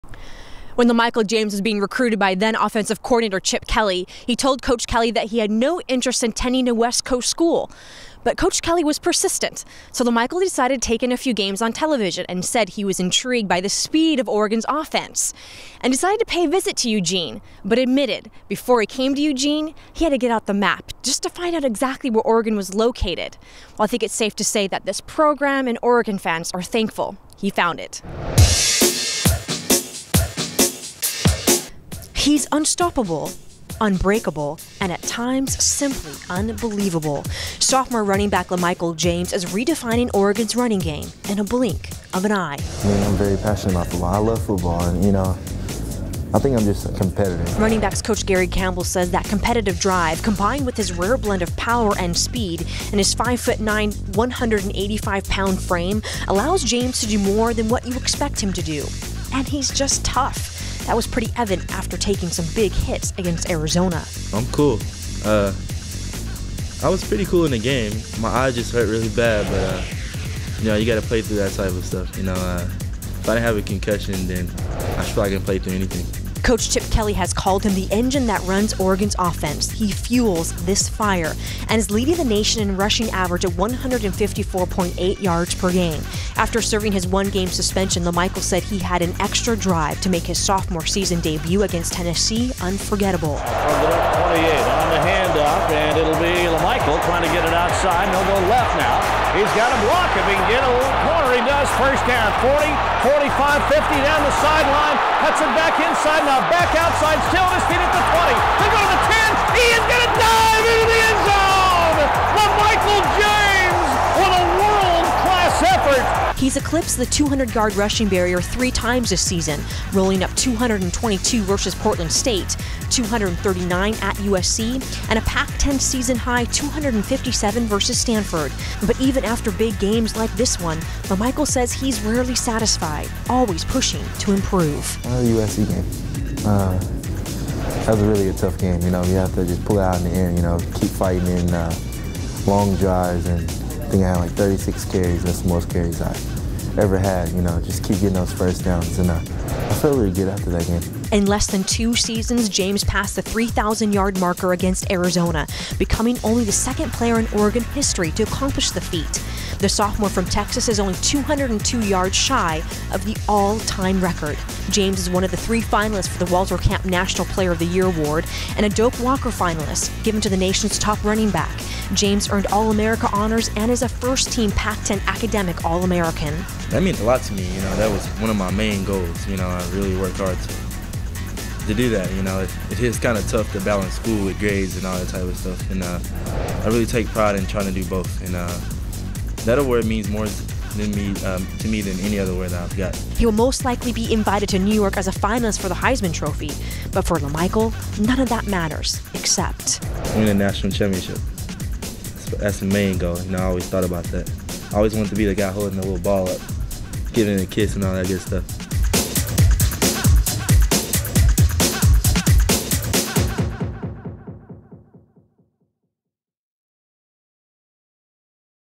LaMichael James
An in-depth look at LaMichael James before the 2010 Civil War. Nicole Abeyta reports.